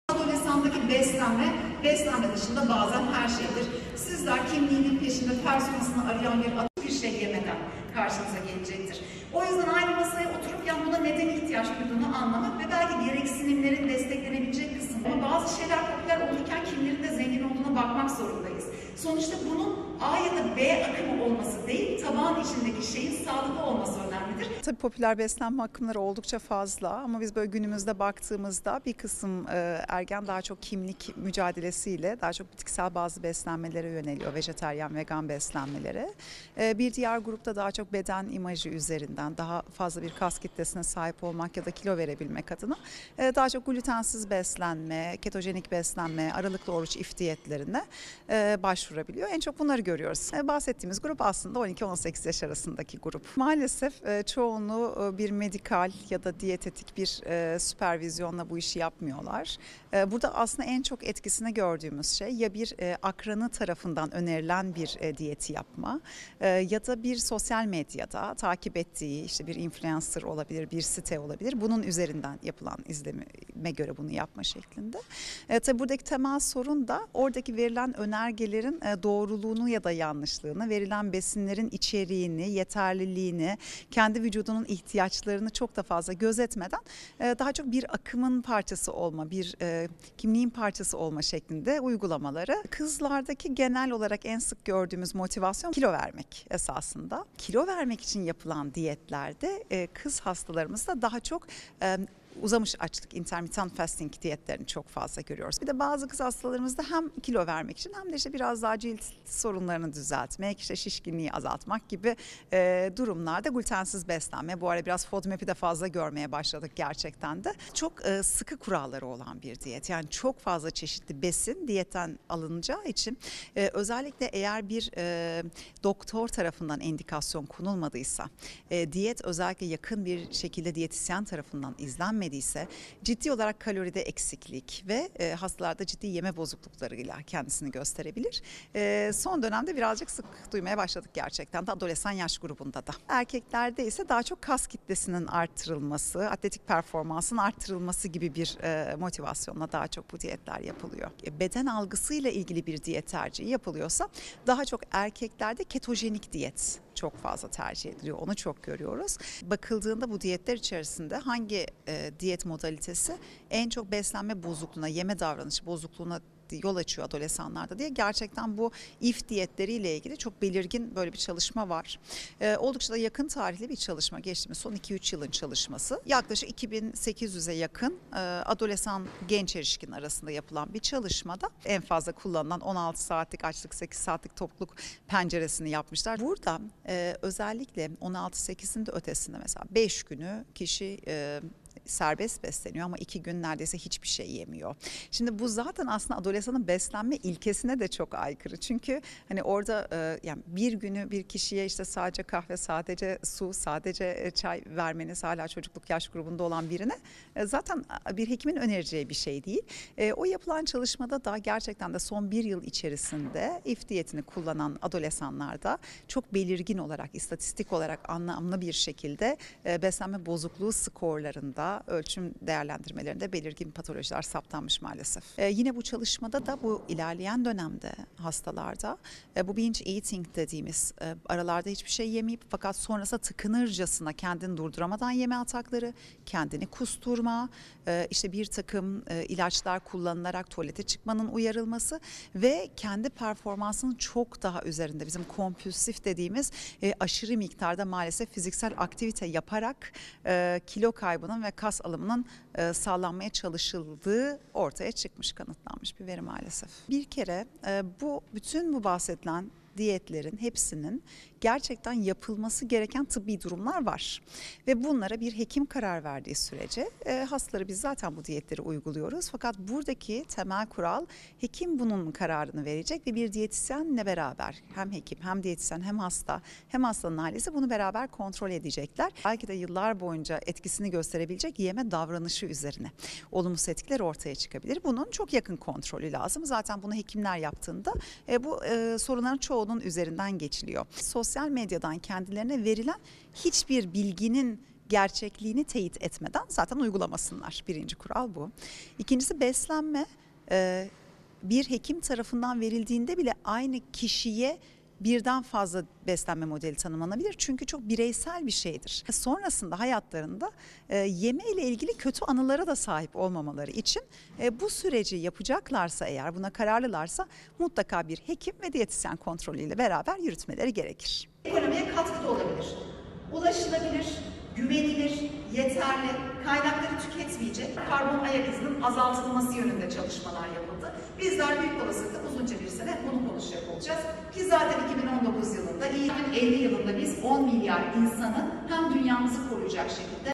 Ergenlerin kapıldığı 'popüler diyet akımları' yeme bozukluğuna yol açabilir
Cerrahpaşa Tıp Fakültesi, Çocuk Sağlığı ve Hastalıkları Ana Bilim Dalı'ndan Çocuk Metabolizma ve Beslenme Uzmanı Doç. Dr. Tanyel Zübarioğlu, daha çok akranlarından ve sosyal medyadaki influencerlardan etkilenerek diyet akımlarına kapılan ergenleri bekleyen sağlık tehlikeleri konusunda uyardı. Doç. Dr. Zübarioğlu, özellikle kızlar arasında çok moda haline gelen ve uzamış açlık veya aralıklı oruç olarak bilinen Intermittent-Fasting (IF) diyetinin uzman gözetiminde yapılmadığında kilo vermek şöyle dursun, yeme bozukluklarına dahi yol açabildiğinin bilimsel çalışmalarla gösterildiğini söyledi.

Türk Pediatri Kurumu'nun (TPK) 22-26 Mayıs tarihleri arasında KKTC'de gerçekleştirdiği 59'uncu Türk Pediatri Kongresi'nde, İstanbul Üniversitesi-Cerrahpaşa, Cerrahpaşa Tıp Fakültesi, Çocuk Sağlığı ve Hastalıkları Ana Bilim Dalı'ndan Doç. Dr. Tanyel Zübarioğlu, ergenler arasındaki 'Popüler Beslenme Akımları' konusunda bilimsel bir sunum gerçekleştirdi. Demirören Haber Ajansı'na önemli açıklamalarda bulunan Doç. Dr. Zübarioğlu, adölesan yani 12-18 ergen yaş grubunun en çok, akran önerisi ya da sosyal medya influencerlarının etkisiyle diyet akımlarına yöneldiğini, kızların daha çok uzamış açlık veya aralıklı oruç olarak bilinen IF diyeti ile bazı besinlerin katı bir şekilde kısıtlandığı Fodmap diyeti ve glutensiz beslenmeye rağbet gösterirken, erkeklerinse kas kütlesini artırmak için ketojenik diyetleri tercih ettiğini belirtti. Özellikle son yıllarda moda olan IF diyetinin, uzman kontrolü olmadan yapılmasının yarardan çok tehlike yaratabileceğine dikkat çeken Çocuk Metabolizma ve Beslenme Uzmanı Doç. Dr. Tanyel Zübarioğlu, bu konuda Kanada'da yapılan ve yaklaşık 2 bin 800 ergenin takip edildiği araştırmanın detaylarına da değindi. Doç. Dr. Zübarioğlu, “Son 1 yıl içinde IF diyetini kullanan ergenlerde belirgin bir şekilde beslenme bozukluğu skorlarının arttığı görülmüş. İlerleyen dönemlerde ise aralarda hiçbir şey yemeyip beslenme döneminde tıkınırcasına, kendini durduramadan yeme atakları, kendini kusturma, ilaçlarla tuvalete çıkmanın uyarılması ve kapasitesinin çok daha üzerinde kompülsif yani mantık dışı derecede fiziksel aktivite yaparak kilo kaybı ve kas kütlesi artışı sağlanmaya çalışıldığı ortaya çıkmış" dedi.

Özlem YURTÇU KARABULUT - Güven USTA / İSTANBUL, (DHA)